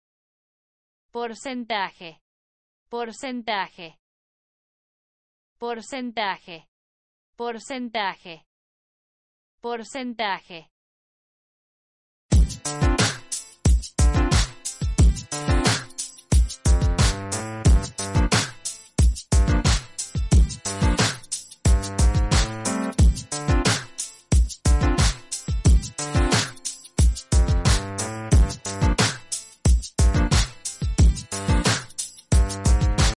How do you say 'percentage' in Spanish?
How to say 'percentage' in Spanish? Listen to hear the pronunciation.